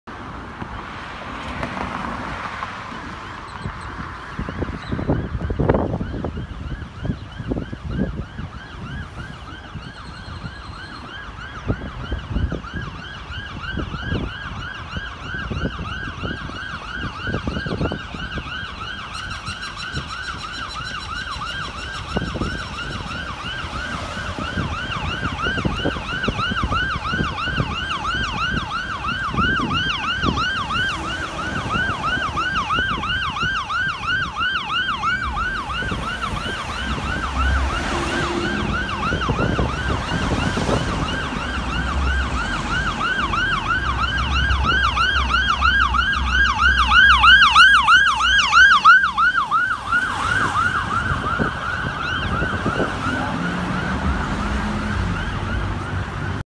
Shell Oil EMS Ambulance#2 en-route to Deer Park for Mutual aid due to our Medics out on other calls
Shell Oil EMS Ambulance#2 en-route to Deer Park for Mutual aid due to our Medics out on other calls, also vehicles not giving Right of Way!!